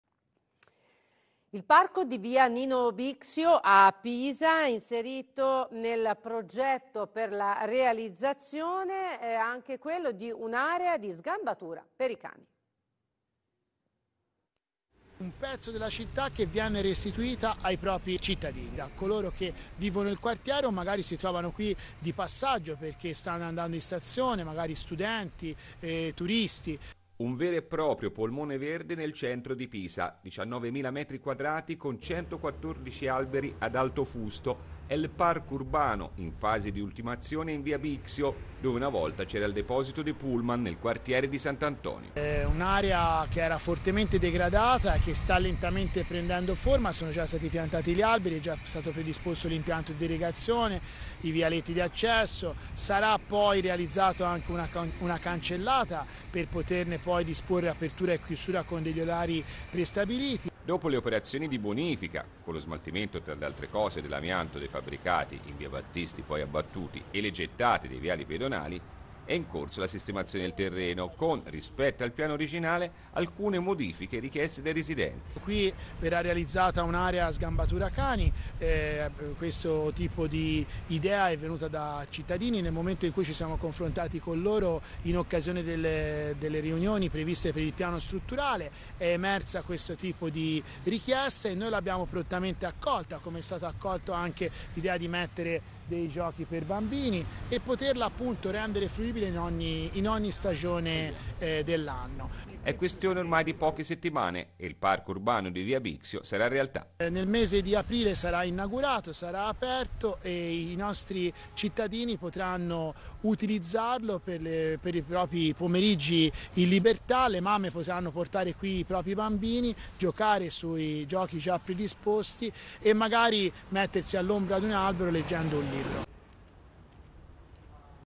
Parco di via Bixio, inserita nel progetto la realizzazione di un’area sgambatura per cani
Sindaco Conti: «Ascolto delle esigenze dei cittadini per integrare e migliorare i progetti in corso d’opera»

Proseguono i lavori nel cantiere che sta realizzando il nuovo Parco urbano tra via Nino Bixio e via Cesare Battisti a Pisa. Grazie ad un’importante attività di confronto tra l’Amministrazione Comunale e la cittadinanza, è stata inserita nel progetto in corso la realizzazione di un’area sgambatura per cani, prevista all’estremità ovest del parco, lato Bastione Stampace.

“Questa modifica è il risultato del nostro metodo di lavoro che prevede un contatto costante con il cittadino – dichiara il Sindaco di Pisa Michele Conti -. Dalle numerose occasioni di confronto con i residenti del quartiere, abbiamo recepito la necessità di realizzare un’area di sgambatura per i cani e siamo subito intervenuti per integrare il progetto in corso d’opera. Tra poche settimane il nuovo Parco di via Bixio sarà realtà: un grande intervento di rigenerazione urbana in una zona rimasta per molti anni in stato di abbandono e degrado, che sta trasformando un ex deposito di autobus in un nuovo polmone verde che ricucirà l’area compresa tra la Stazione, la Sesta Porta, l’inizio del quartiere di Porta a Mare e l’ingresso dell’asse pedonale in centro storico. Un parco urbano a disposizione del quartiere che crea un collegamento diretto fra i due assi viari di via Battisti e via Bixio. Un luogo di aggregazione per i cittadini, con area giochi dedicata ai bambini, area sgambatura per cani e spazi come una piccola piazza per il ritrovo e la socialità.”